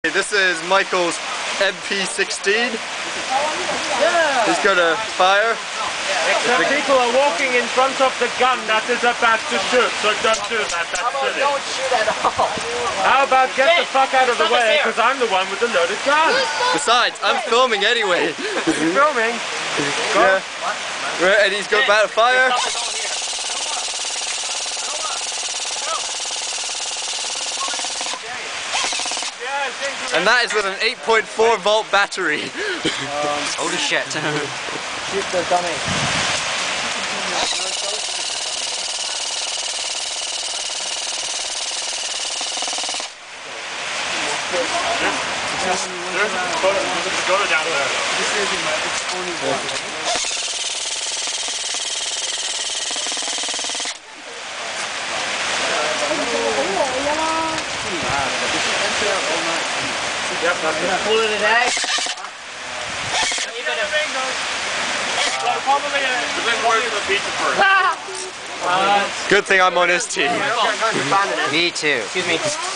airsoft gun mod MP16
simply an MP5 with an M16 lower reciever and god knows how many upgrades.